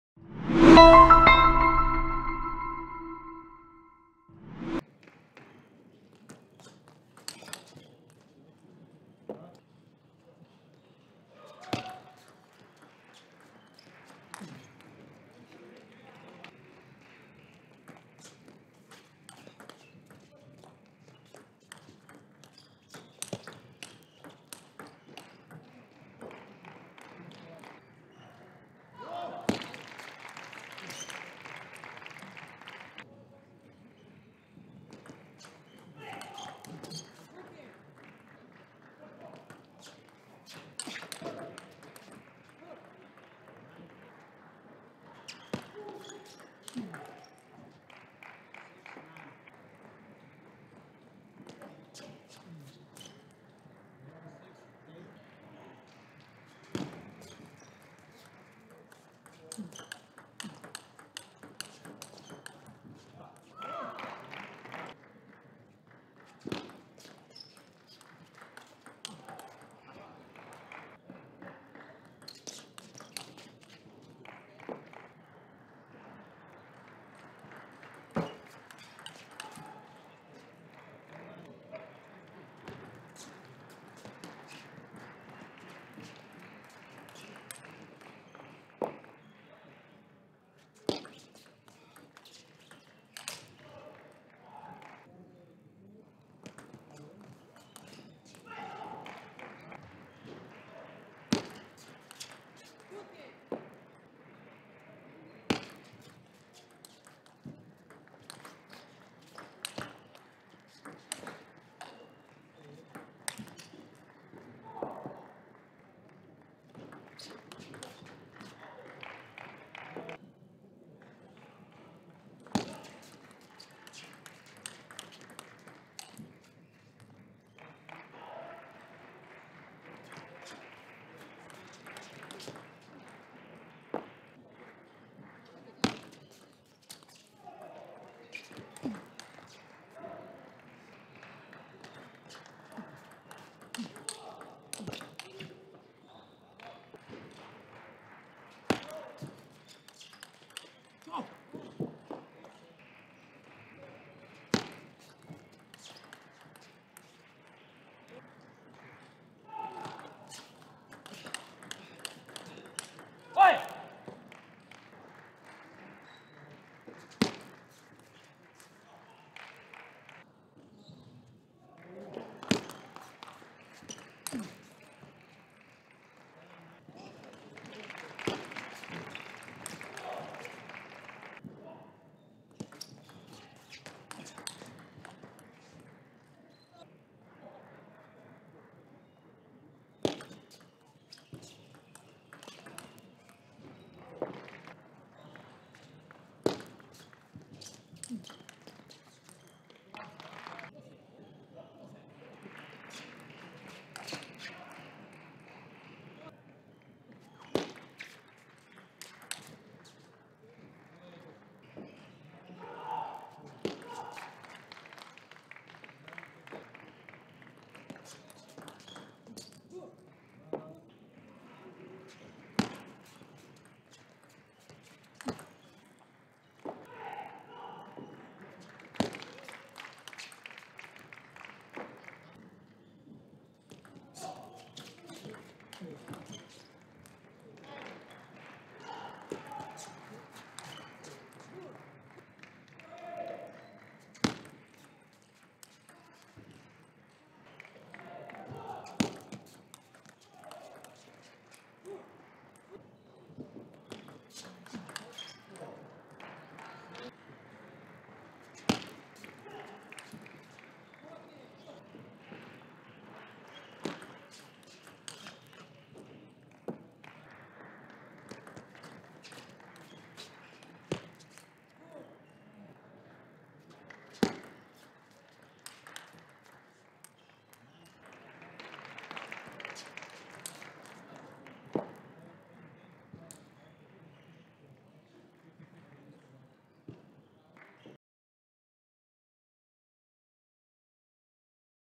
Anton Kallberg vs Kou Lei | 2020 ITTF Hungarian Open Highlights (FS)
Review all the highlights from the Anton Kallberg vs Kou Lei at the 2020 ITTF World Tour, Hungarian Open